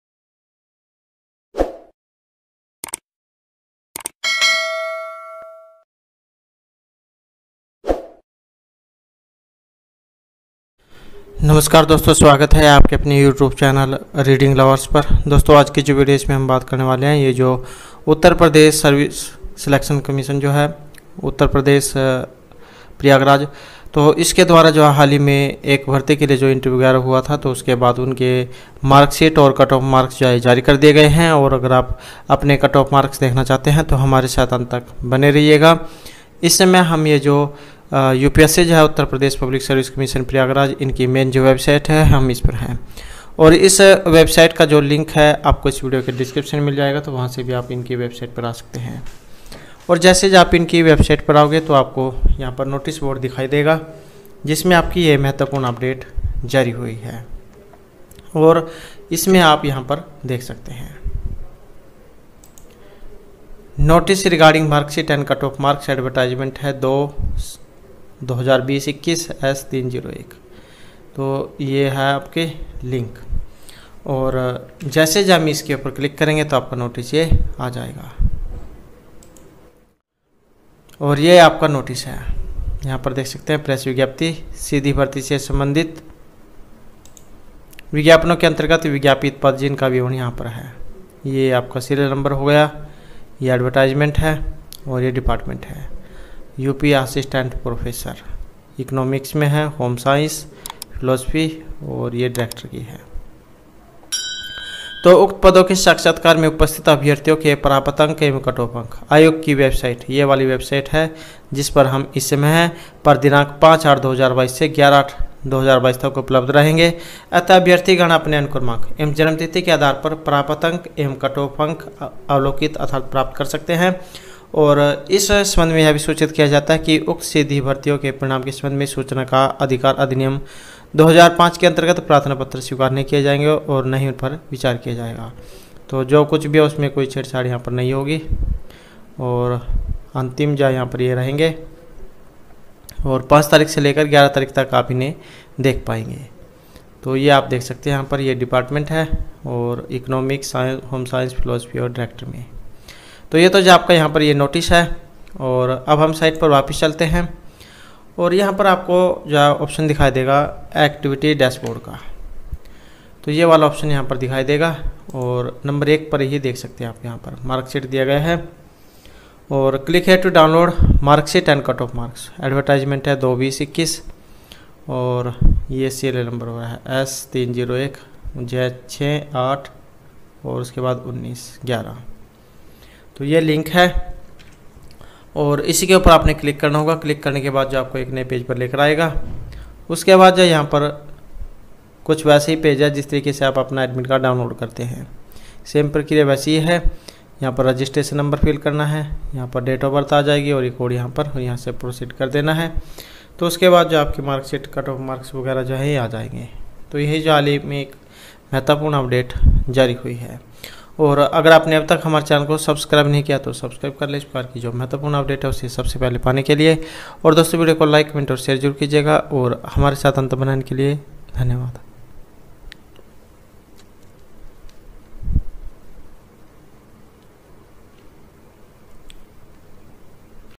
UPPSC latest news today. UP assistant professor marksheet & cutoff marks . UPPSC new update today
In this video, we'll discuss How to download  UPPSC -NOTICE REGARDING MARKSHEET & CUT OFF OF ADVT. NO. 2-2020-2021 [S-3-01], [S-3-06], [S-3-08] & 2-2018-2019 [S-3-11].   It's an amazing opportunity for all Indian candidates to get a government job in up.

TAGS.